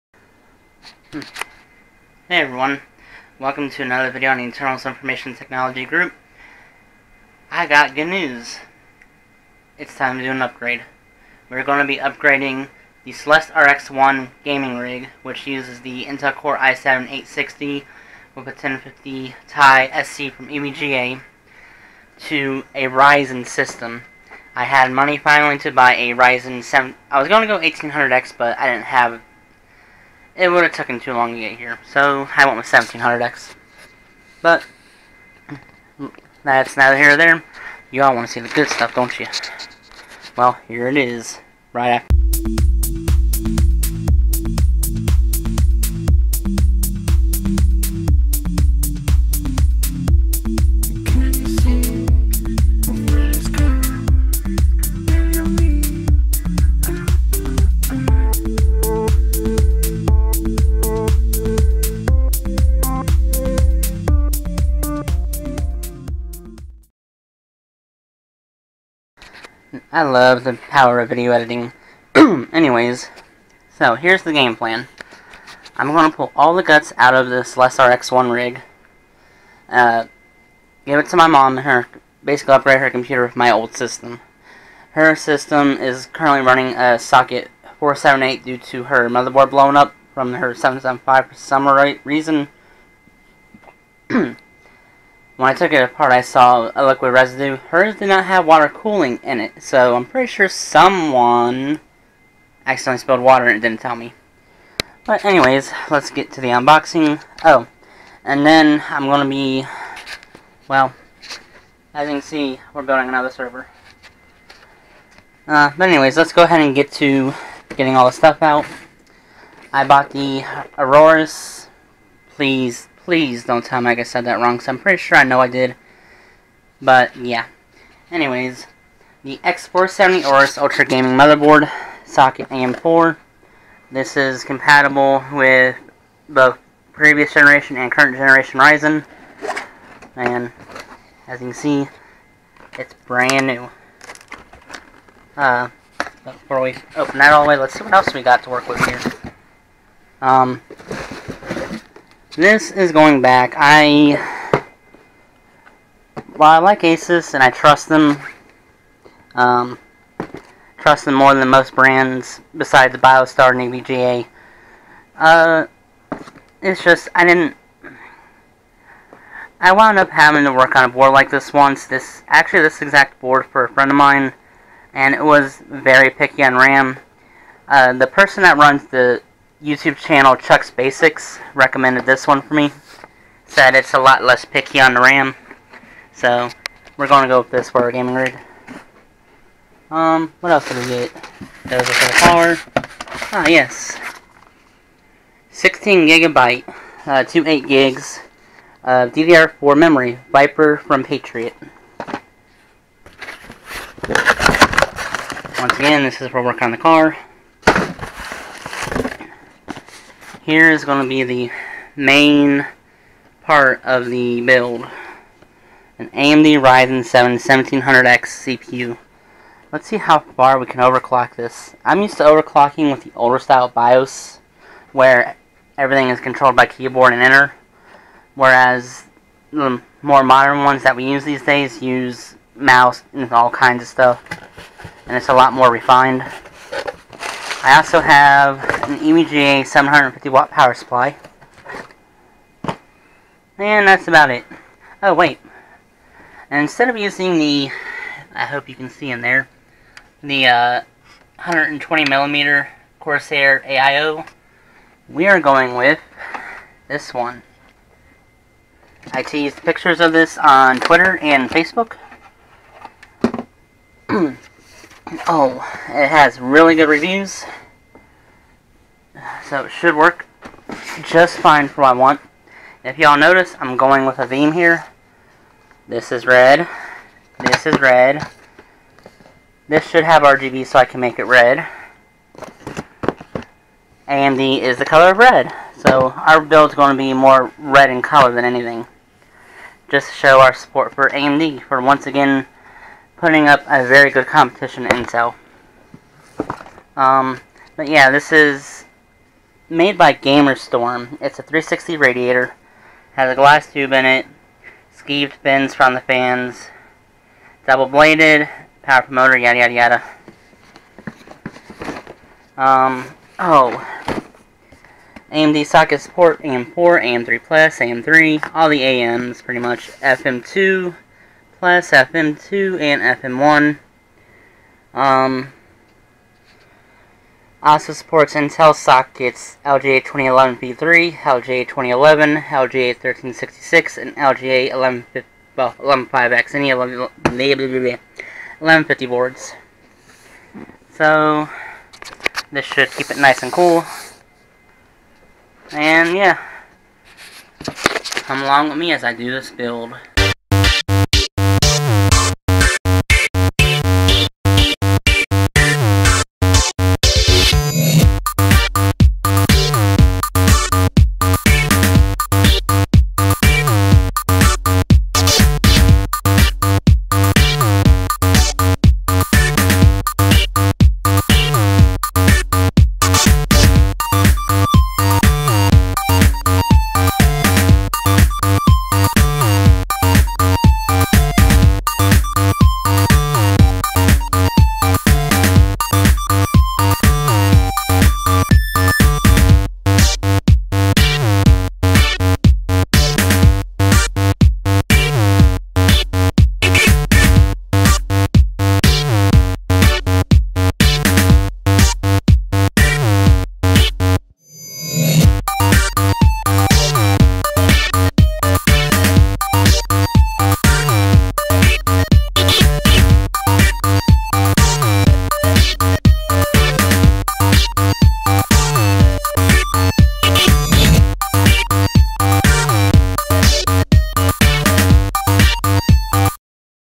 EITG Unboxing of My New Ryzen System Hardware
The Video of me Building it will be the next video I post, which will be next week.

MoBo: GIGABYTE X470 AORUS

CPU Cooler: DEEPCOOL Captain 360EX

RAM: Patriot Viper Elite Series DDR4 16GB (2x8GB)

PSU: EVGA 750 N1, 750W